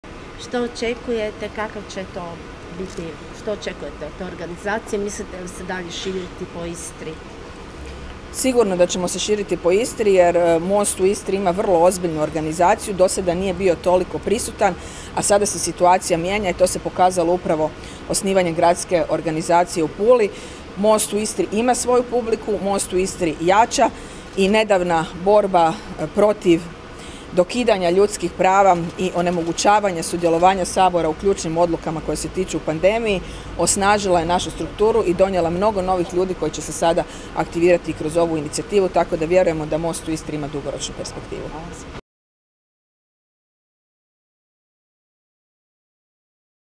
Marija Selak Raspudić: "Most u Istri ima vrlo ozbiljnu organizaciju"
Na konferenciji za medije Kluba zastupnika Mosta održanoj U Ćirilimetodskoj ulici o temi „Ekocid“ govorile su zastupnica Marija Selak Raspudić i profesorica Sunčana Roksandić.